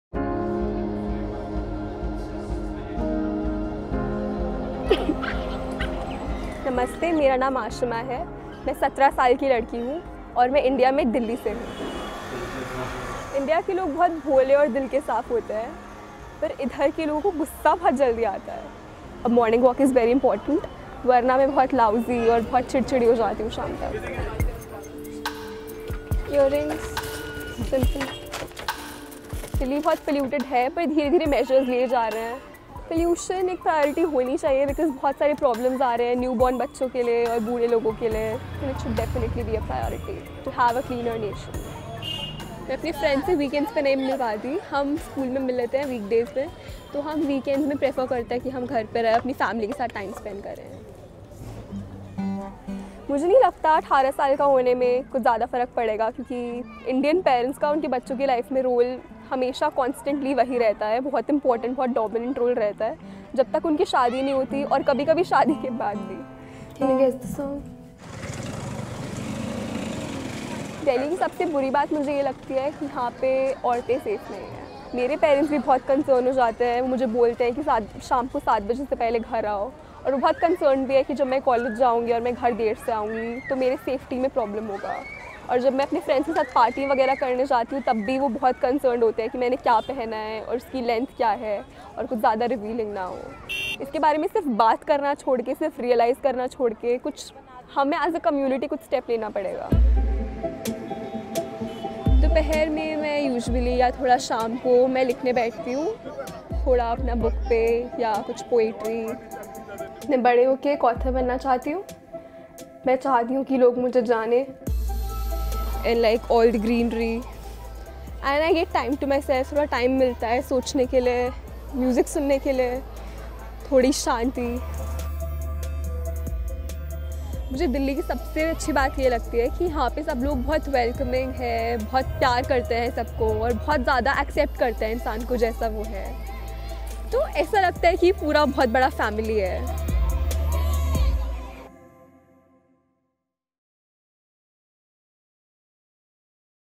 Аёл ва Ҳиндистон: Деҳлида 17 ёшли қиз бўлиш қандай? - BBC Uzbek
Ҳиндистонда 17 ёшли қиз бўлиш нимани англатади? Бу осонми ё қийин? Хавфлими ё хавфсиз?